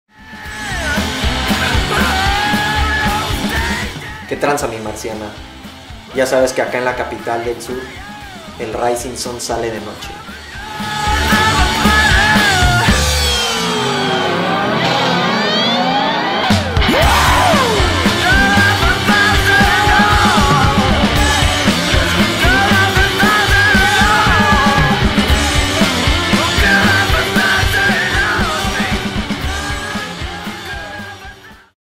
Rael te invita a La Escena Apesta
Rael te da unos tips para que vengas con toda la actitud y estilo a LaEscenaApesta. Recuerda, 27 de abril, Jardin Arcadia Cuernavaca.

"Somos el ruido en tu cabeza."